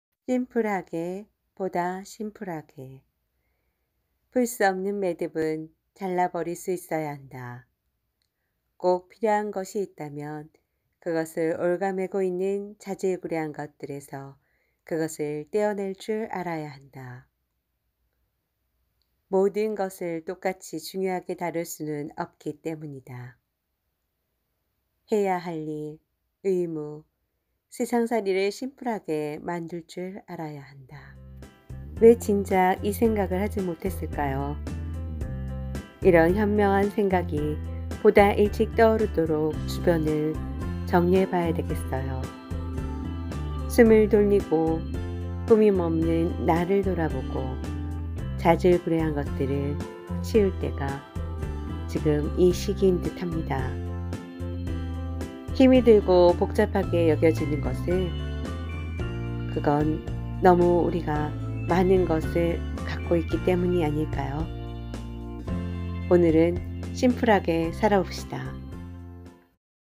심플하게 보다 심플하게-좋은글
심플하게 보다 심플하게 좋은글로  복잡하게 얽혀있는 실타래를 풀지 못하는것은 많은것을  똑같이 잘하려는 마음에서  생겨날수 있습니다.

복잡함을 정리하는 시간을 가져보기 바랍니다.